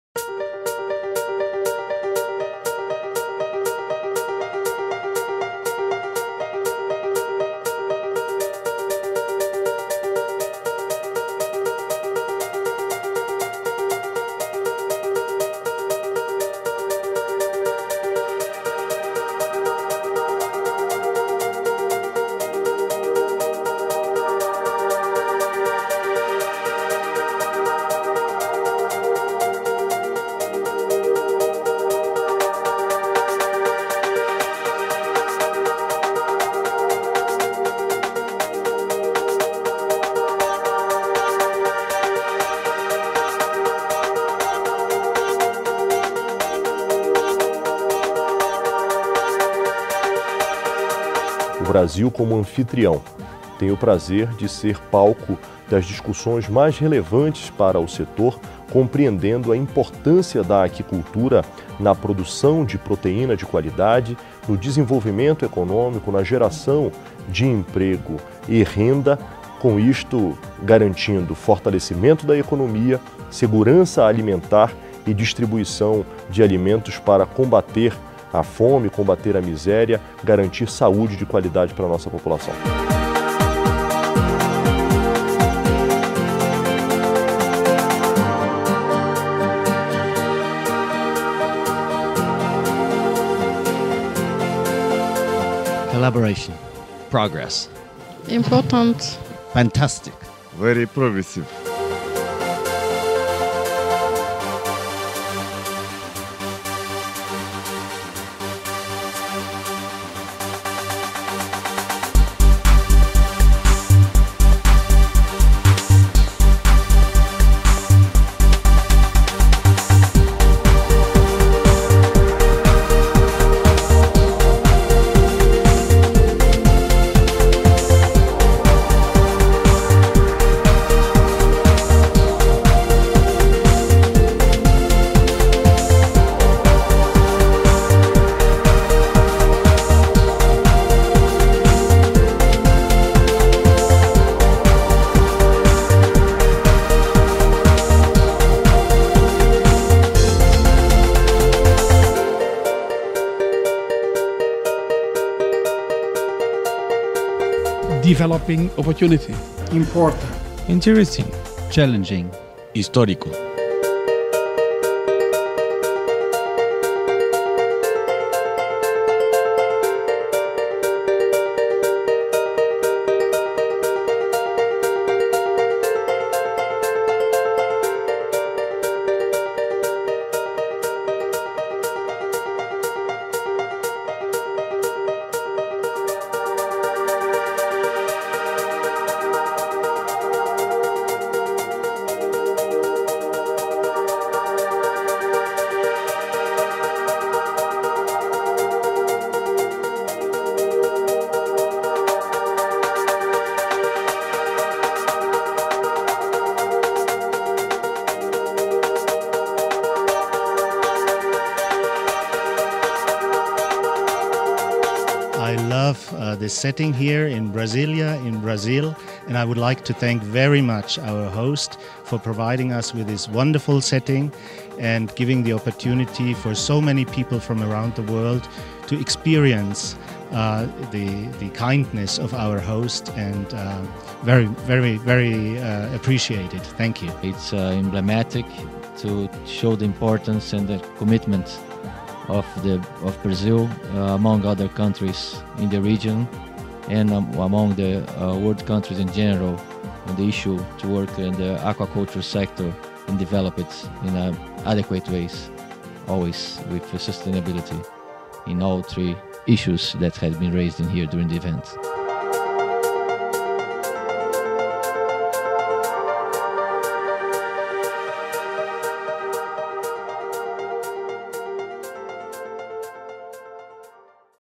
Sub-committee on Aquaculture Brasilia, Brazil 05-09 October, 2015
The adoption of the report with major recommendations for global aquaculture development marked the closing of the 8th Session of the FAO’s Committee on Fisheries (COFI) Sub-Committee on Aquaculture which was held in the capital of Brazil from 05 to 09 October 2015, attended by 100 delegates from around 50 countries of the Americas, Africa, Asia and Europe which represent the vast majority of aquaculture producers worldwide. The meeting of the 8th Sub-Committee on Aquaculture debated a range of important aspects pertinent to the development of the aquaculture sector. The Sub-Committee supported the efforts of FAO in developing the Technical Guidelines on Aquaculture Certification and the Evaluation Framework to assess the conformity of aquaculture certification schemes with the guidelines. On FAO’s Blue Growth Initiative, it highlighted inter alia the positive effects from aquaculture on the environment such as through shellfish and seaweed farming and requested to increase the number of participating countries in the piloting phase. The Sub-Committee emphasized resource use efficiency in aquaculture for food and nutrition security as a key focus area, recognizing that research and education needs may vary from region to region. It suggested that issues on decent employment could be dealt with by the FAO aquaculture certification guidelines. Strong support was expressed to improve aquaculture data, information and statistics compiled by FAO, including the need to integrate the data collected from the first State of the World Report on Aquatic Genetic Resources into FAO’s fisheries and aquaculture information systems. Regarding aquaculture’s contribution to food and nutrition security, poverty alleviation and national economies, the Sub-Committee recommended that FAO continue its work in this area and provide information on good farming practices, how to assess aquaculture’s contribution to food and nutritional security, and its impact on policy development.